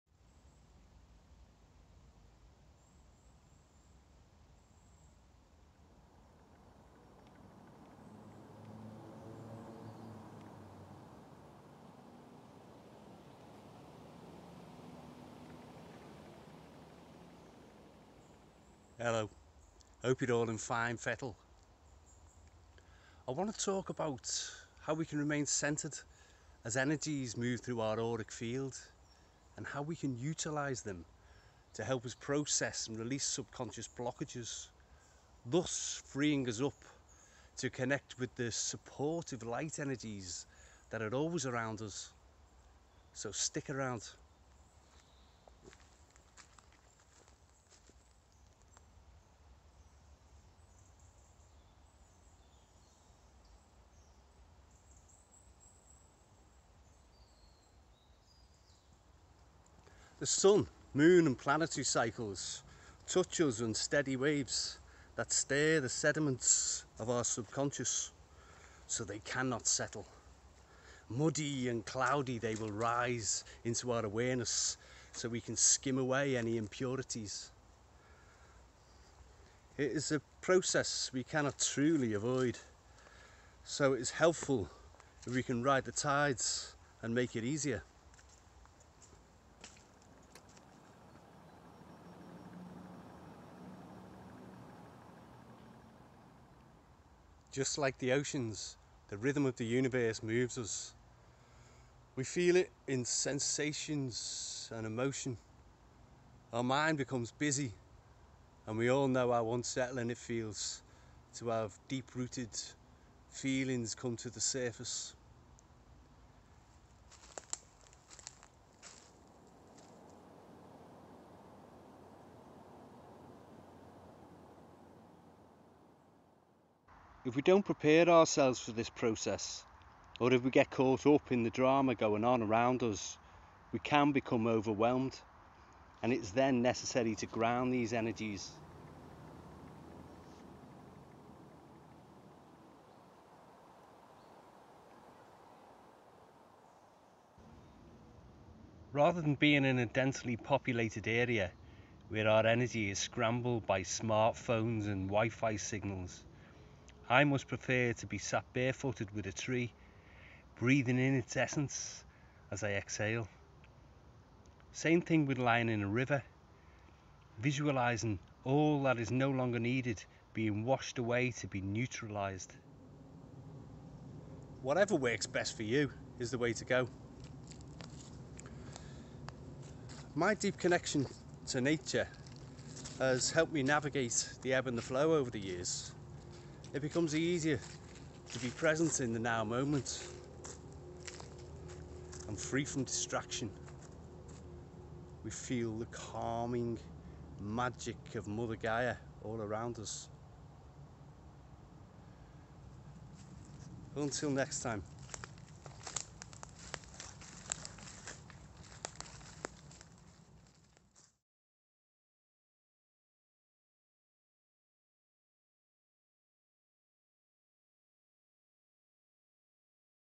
The Auric Field & Our Awareness of Energetic Fluctuations
Walkin'/Talkin', Enjoying the Calming Magic of Mother Gaia. Meditation , Sitting With The Trees and Walking Barefooted . Forest Mushrooms and Closeness To The Earth . Remaining Centered and Grounded